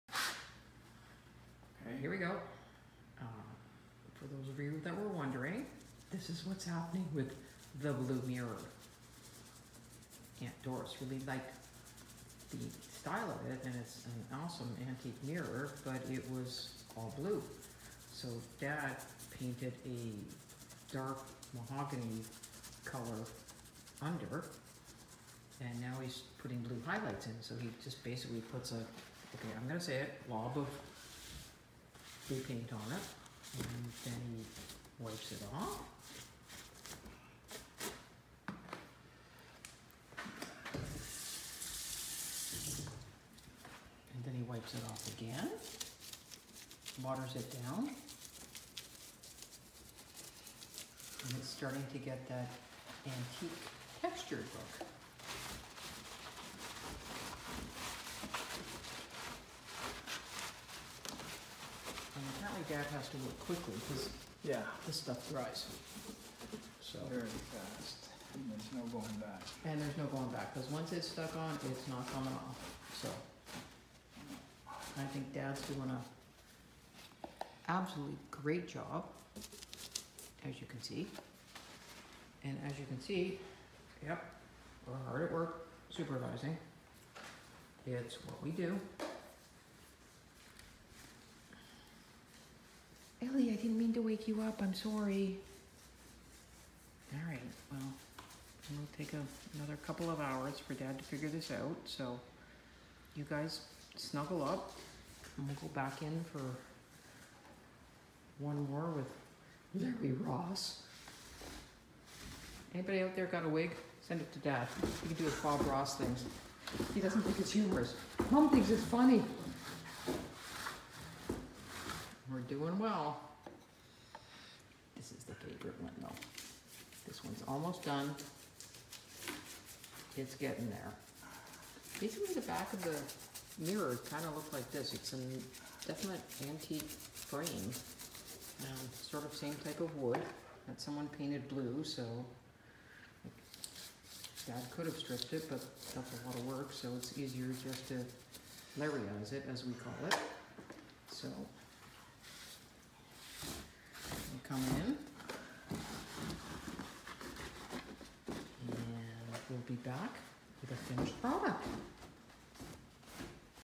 Artist Demonstrates Antique Wash Paint Technique While Great Danes Nap
Mikey and Ellie the Great Danes love to hang out with Dad Lawrence Dyer in his art studio, but aren't overly interested in painting or Dad's tubes of paint, thank goodness. Watch and enjoy as Dad give a quick lesson on how to apply an antique wash finish to a frame that was originally baby blue in colour.